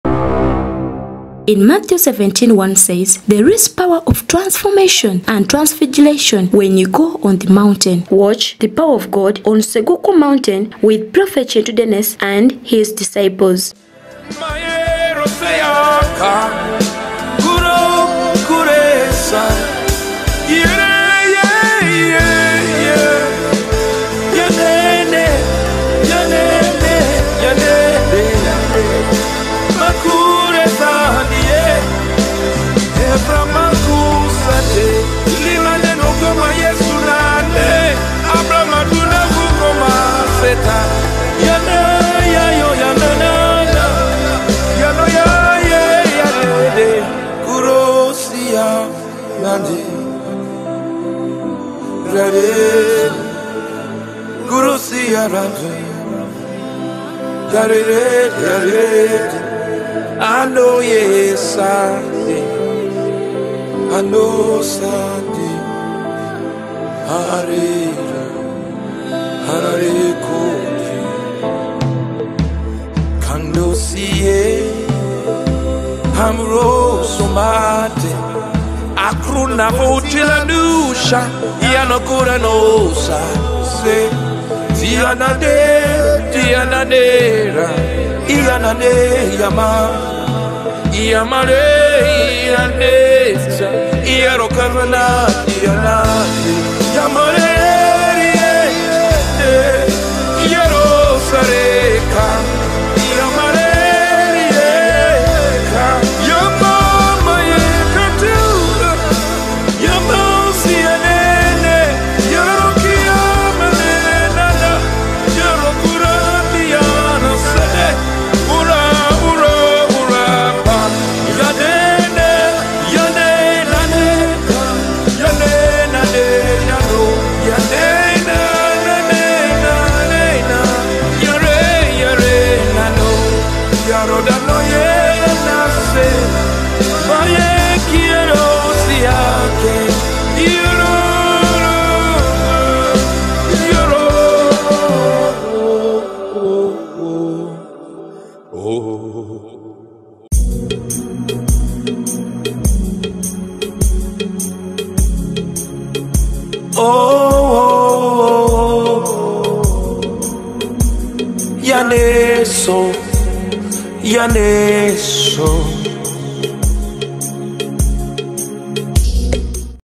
SEGUKU PRAYER MOUNTAIN
MINISTERS ARE BEING ENCOUNTERED BY  THE HOLY SPIRIT